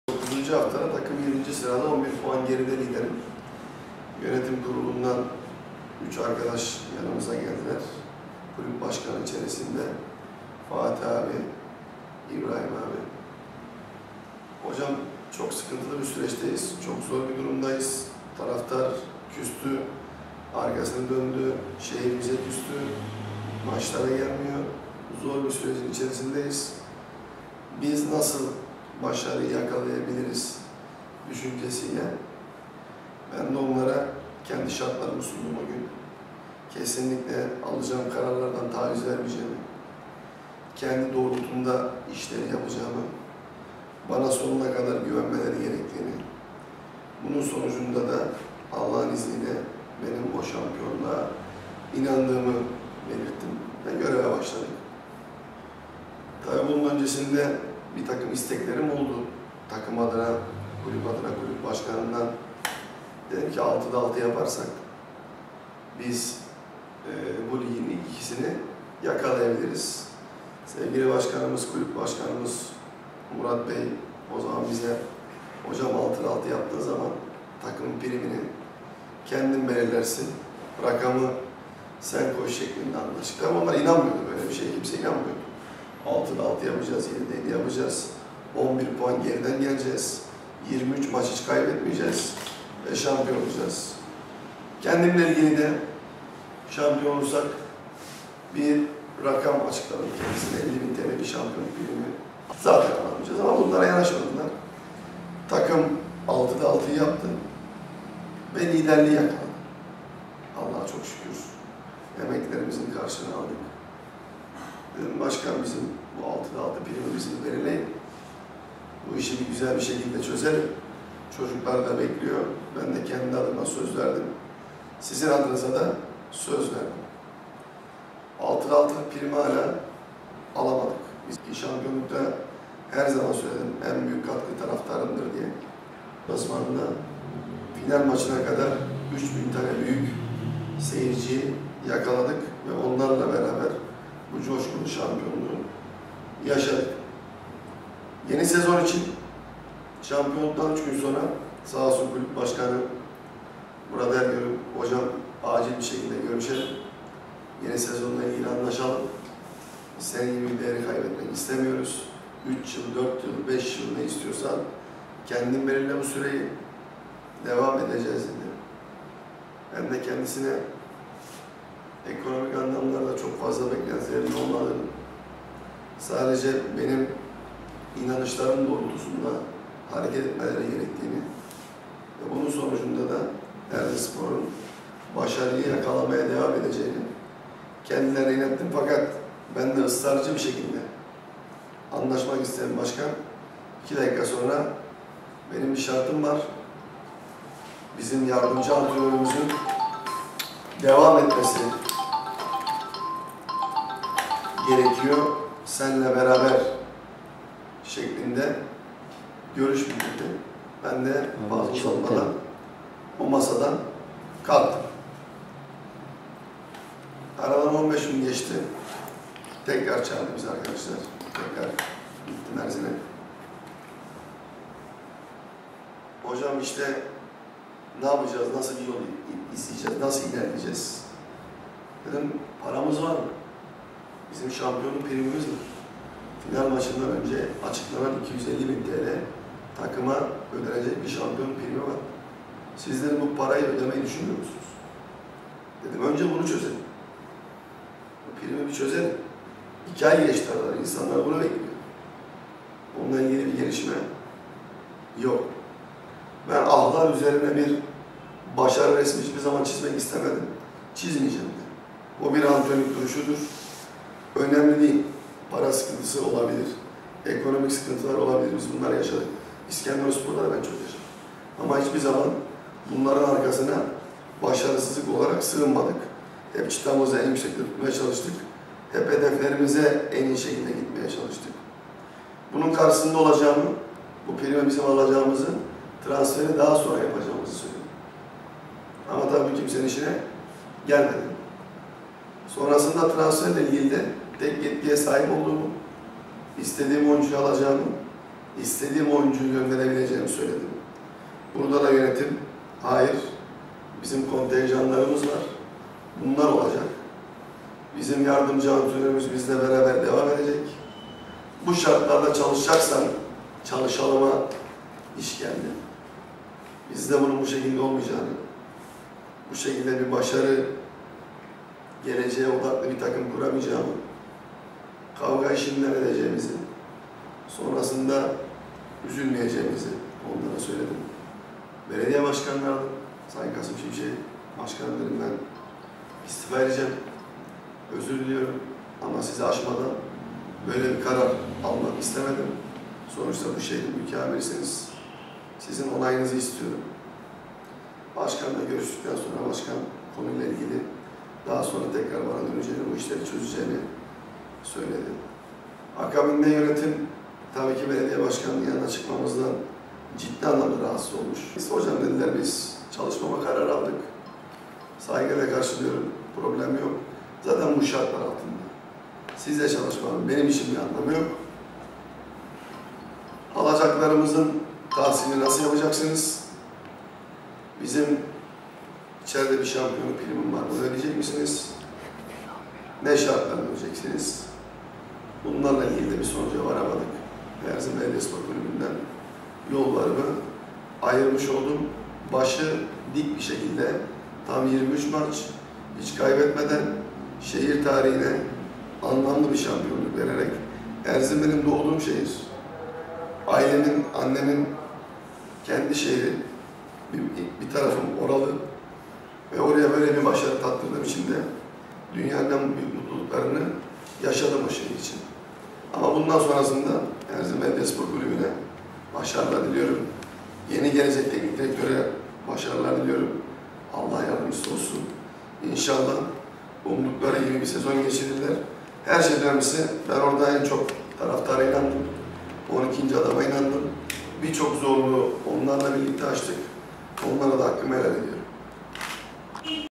Erzin Belediyespor Güven'i Kaybetti!
Erzin Belediyesporu 9 haftada 7 sırada 11 puanla alıp 23 maç kaybetmeden bir şampiyonluk sevinci yaşatan teknik Sorumlu Can Güven başı dik, gönlü kırık bir şekilde takımdan ayrıldığını söyledi.